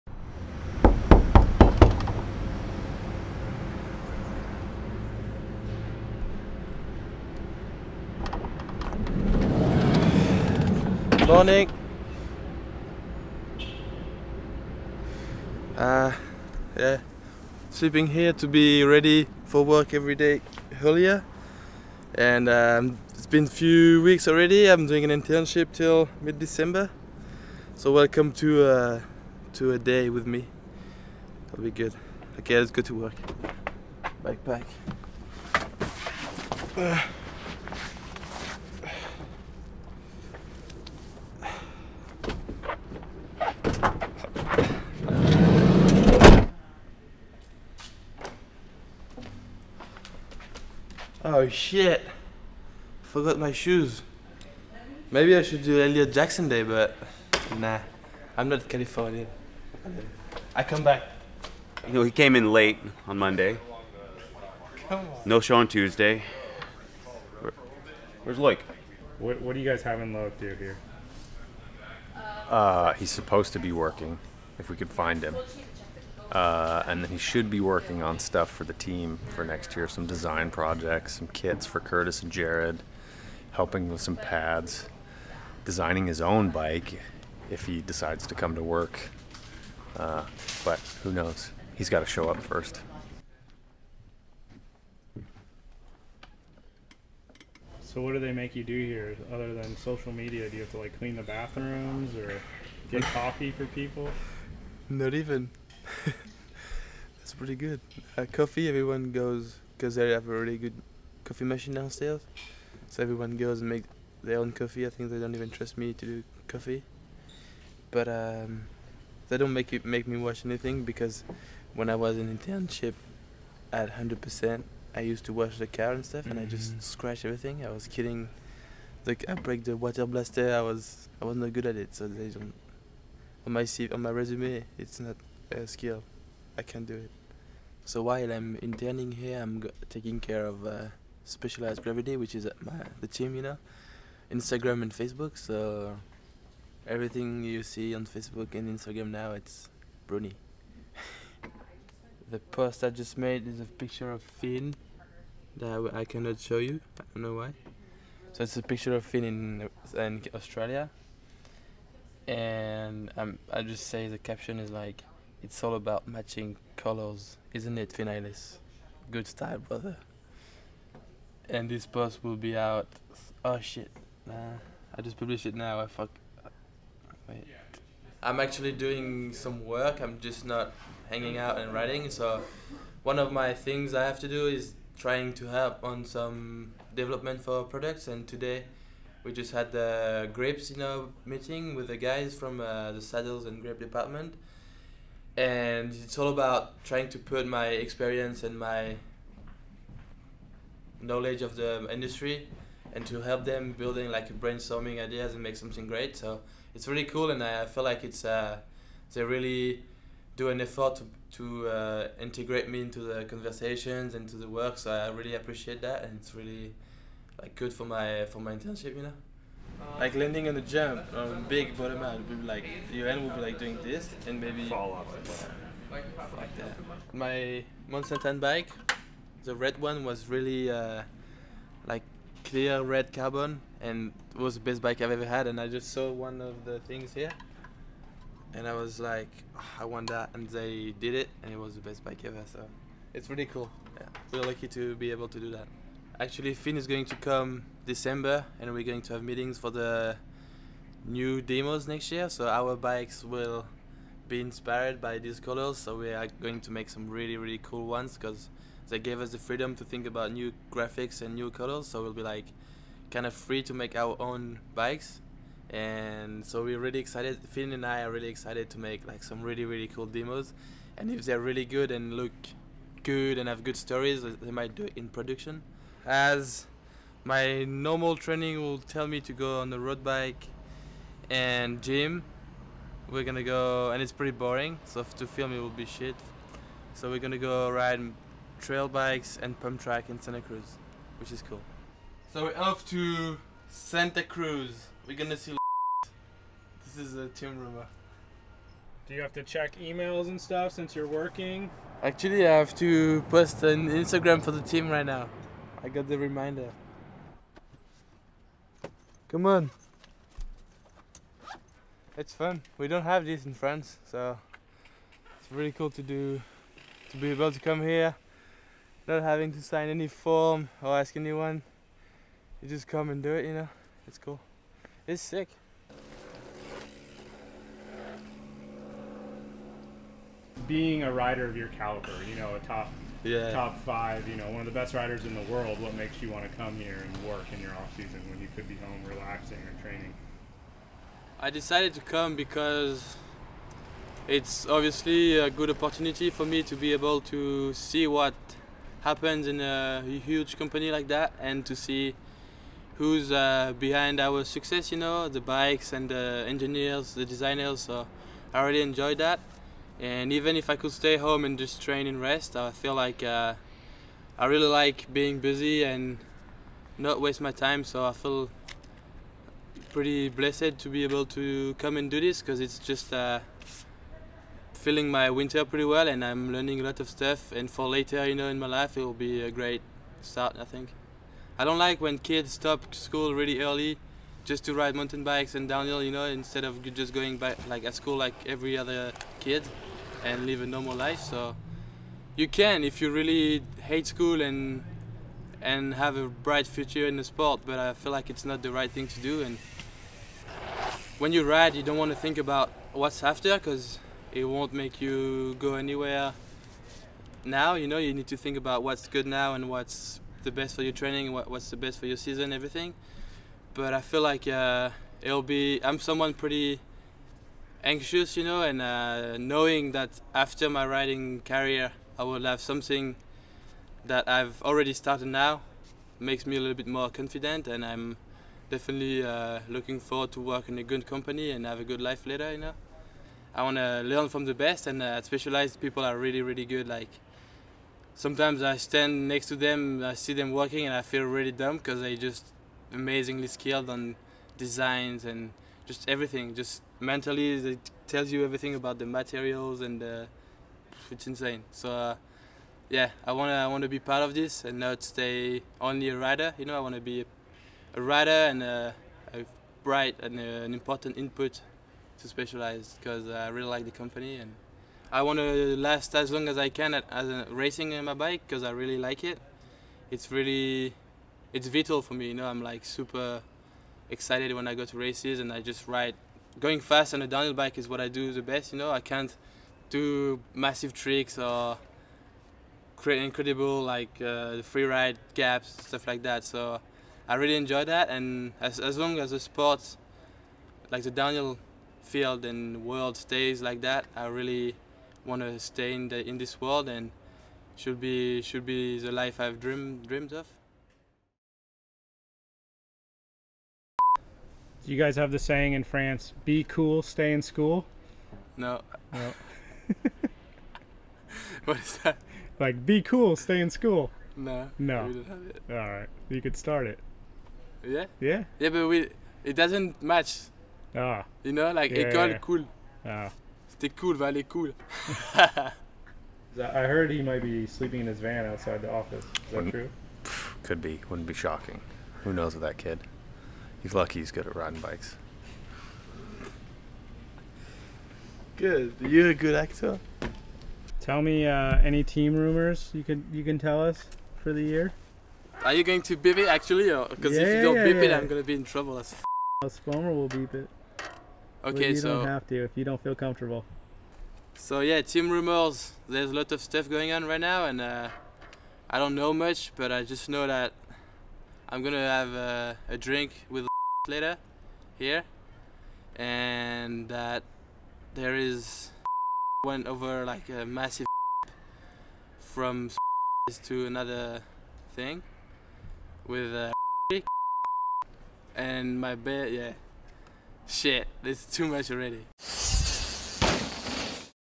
A Day in the Life of Loic Bruni - A Vital MTB Exclusive
One of mountain biking's fastest racers is also one of the funniest and most inspiring. Loic Bruni spent part of his off-season as an intern at Specialized, gaining valuable experience and knowledge for a life after racing.